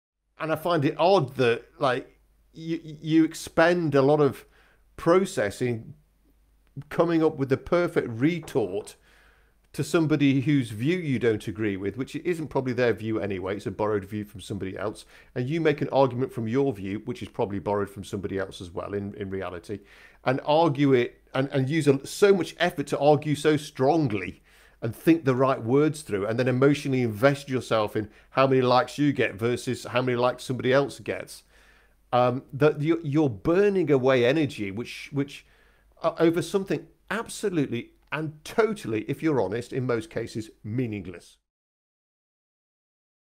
Hacking the brain teaser
Dr David Day is really making us think as he introduces the topic of brain hacking and how vulnerable we are to information sources around us.